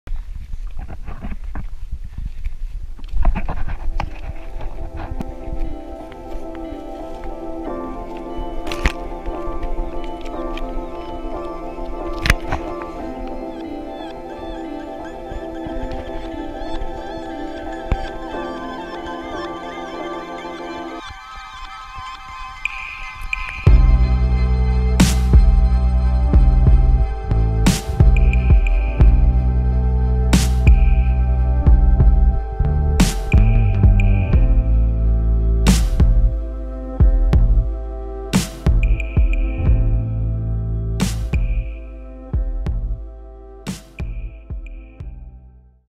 Sur les hauteurs
Belle montée sur les pentes du massif de l’Etale à La Clusaz pour aller admirer les premières neiges ❄️🏔😎